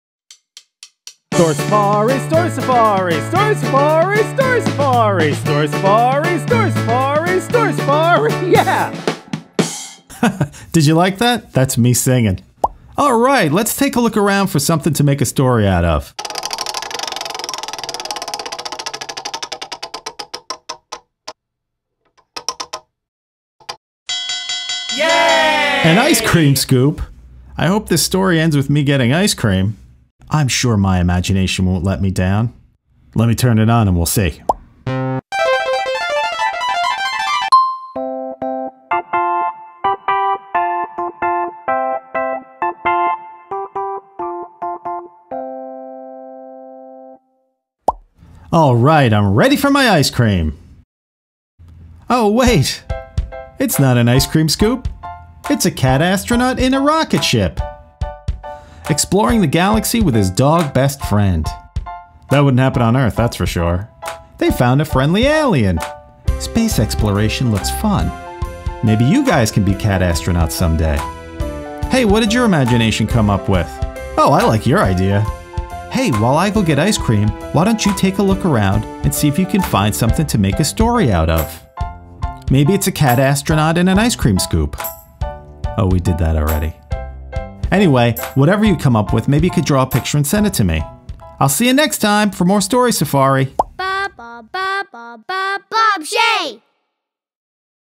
Story Safari Ice Cream Scoop Adventure! Imagination! Fun!
A super simple screen-free activity kids can do at home! Pick a thing and make up a story! Children’s author Bob Shea shows you how.